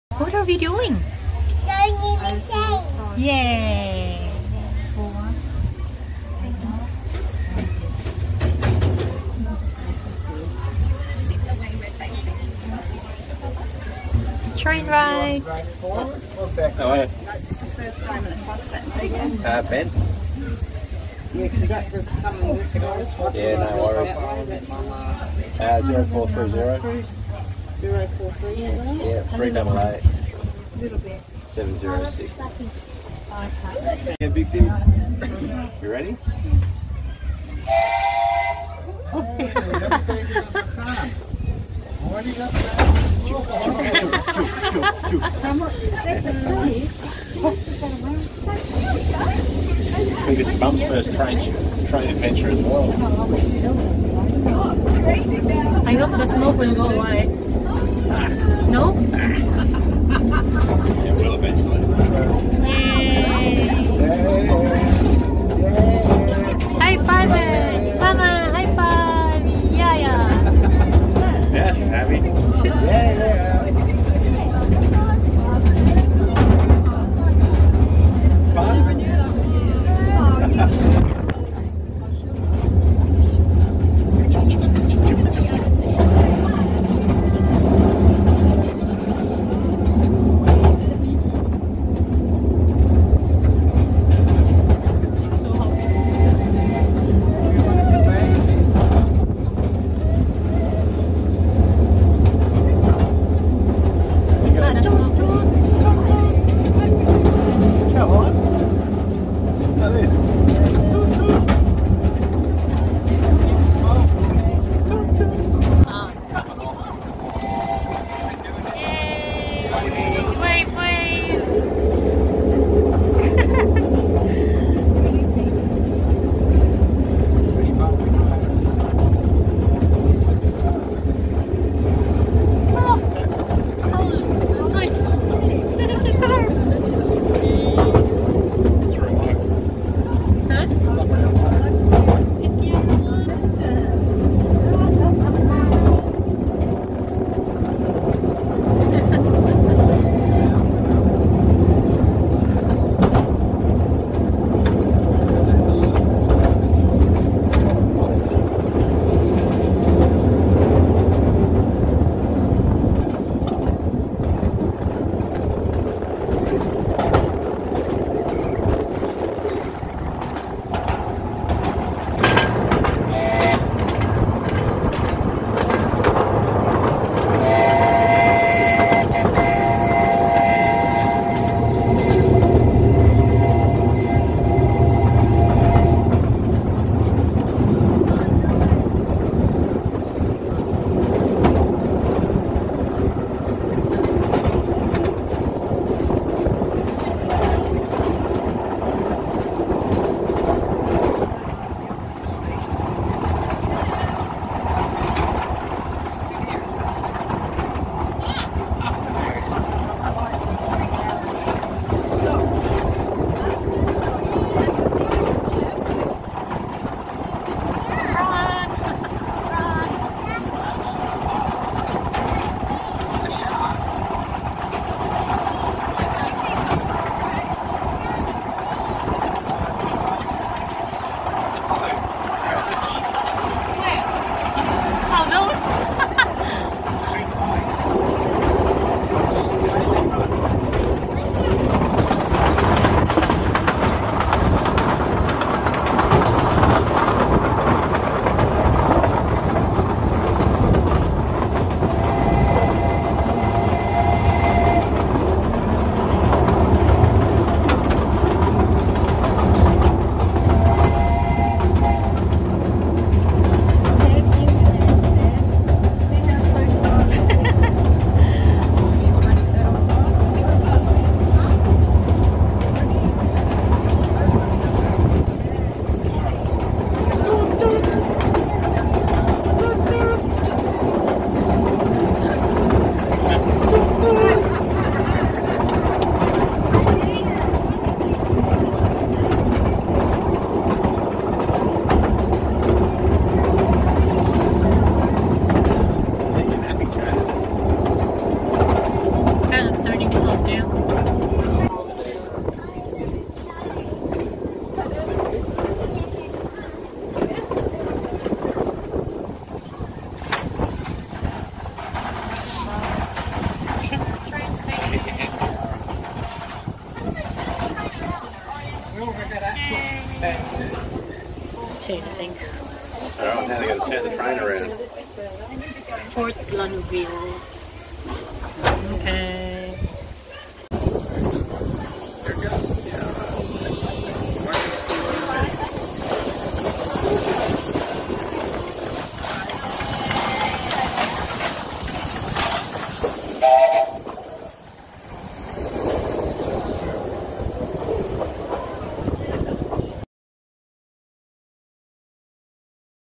Semaphore Steam Train Ride | Family Travellers Official
Riding a steam train at semaphore.